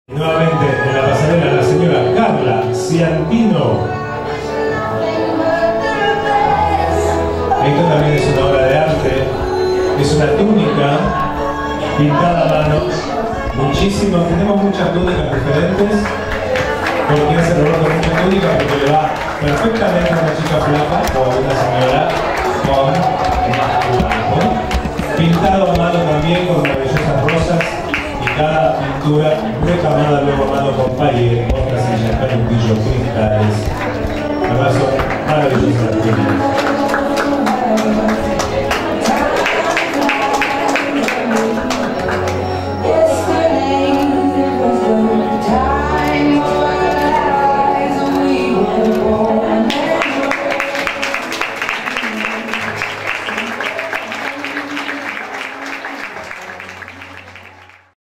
Carla Ciantino (2do Vestido) - Roberto Piazza. Casa Del Teatro.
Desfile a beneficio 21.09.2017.